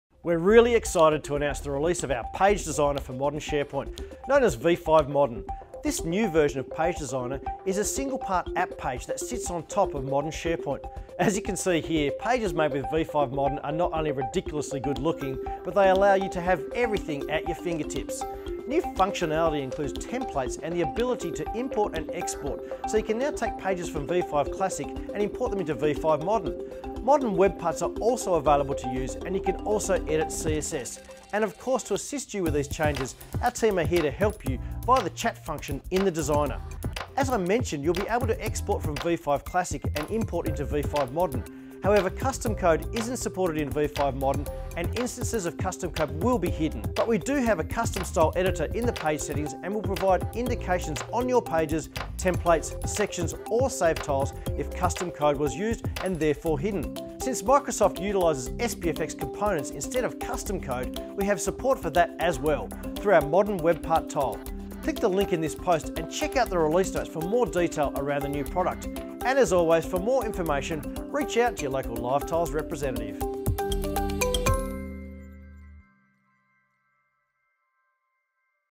LiveTiles Page Designer (Sharepoint) Product Update - V5 Modern
We’re excited to announce the release of our Page Designer for Modern SharePoint, known as V5 Modern. This new version of page designer is a single part app page that sits on top of Modern SharePoint.